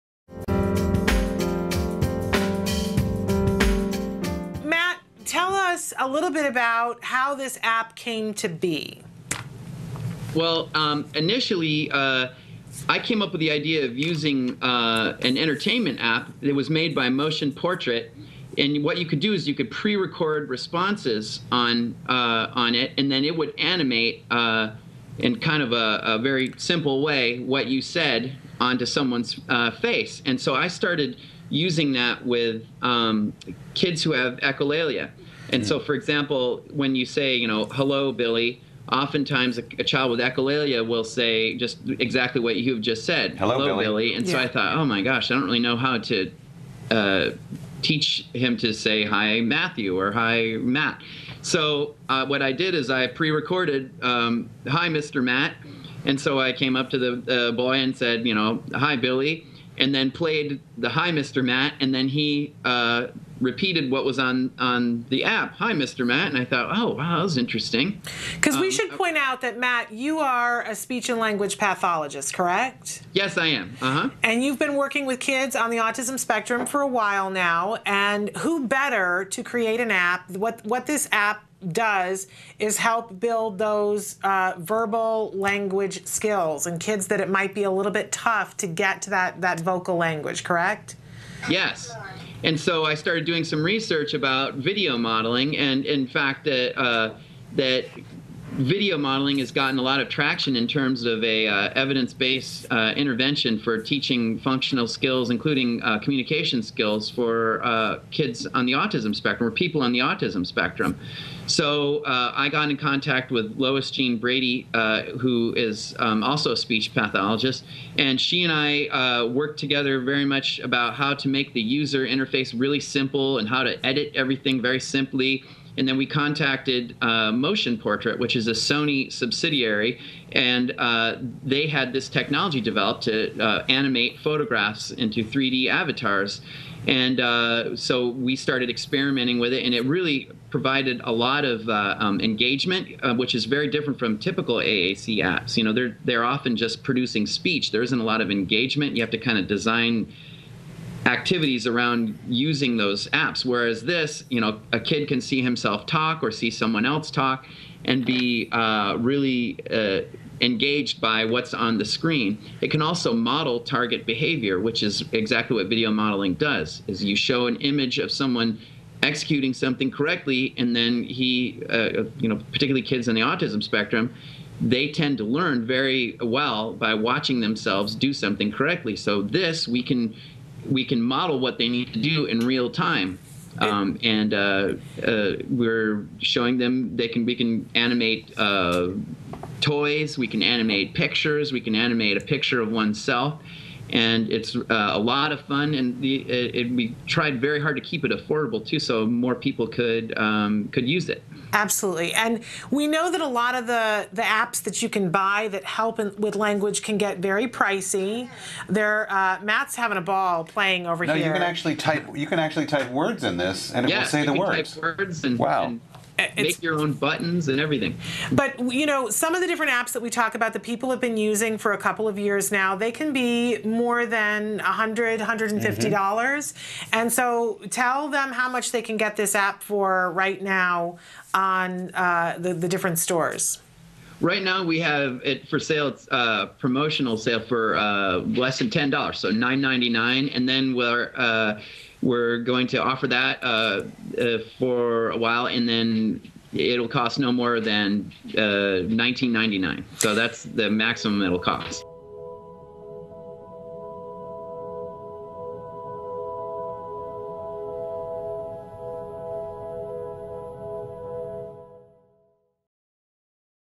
InnerVoice Fun Way to Build Skills
Matt Guggemos, one of the creators of the hot new app InnerVoice, shares this advanced technology which animates pictures in order to show them talking. Children with ASD can watch pictures of themselves speaking to help activate mirror neurons and encourage verbal speech.